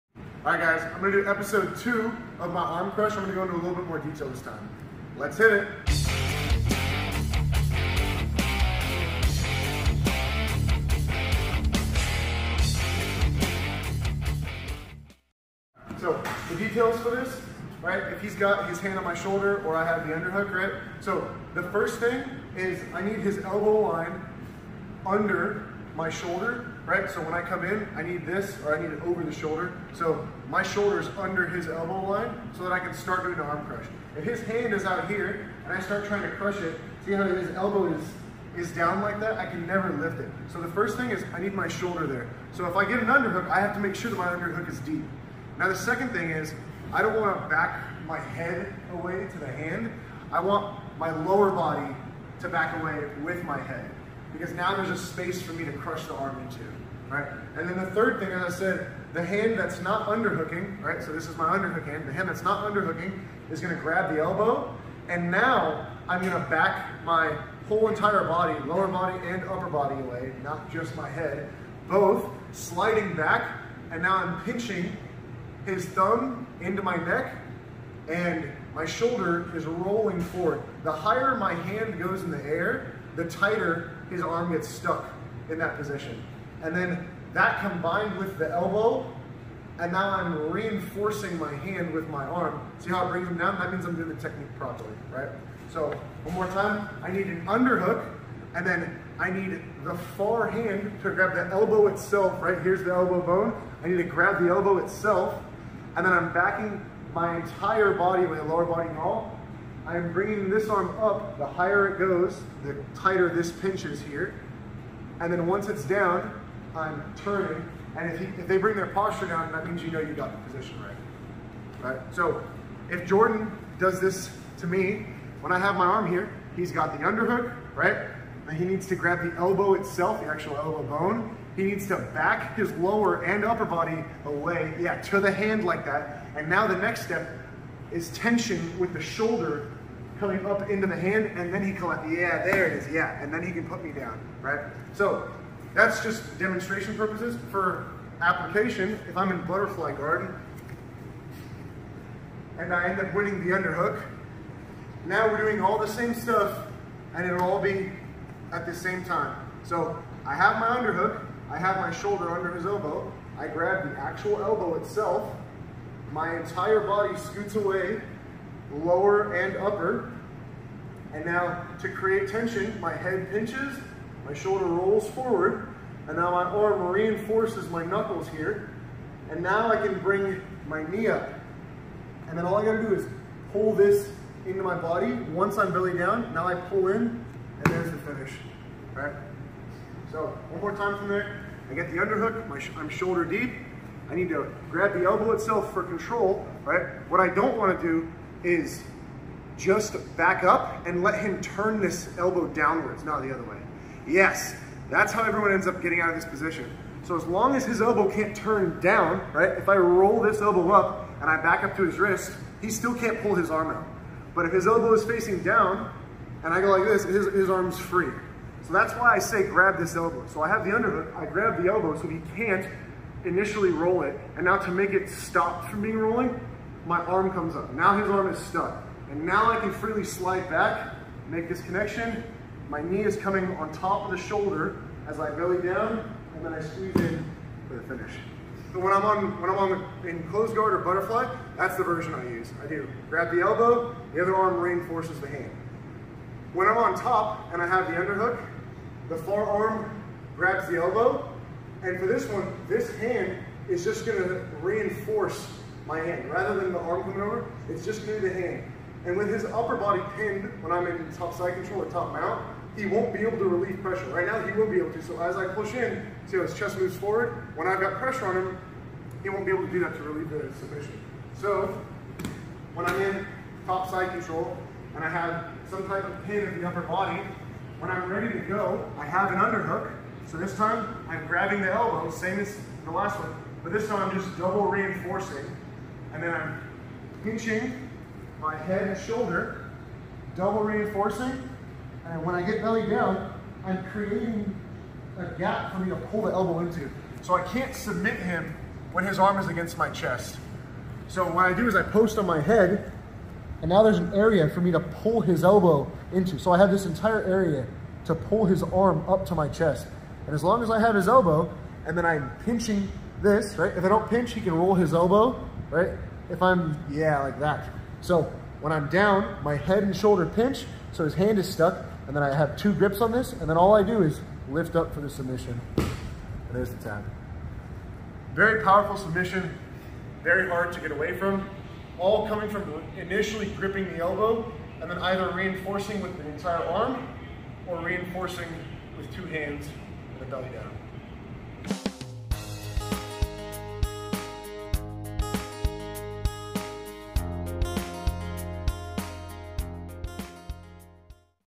Episode 15 - DEADLY Arm Crush Part 2 [White Belt Series] | Brazilian Jiu Jitsu
Jonathan goes over even more details and positions when it comes to this arm crush.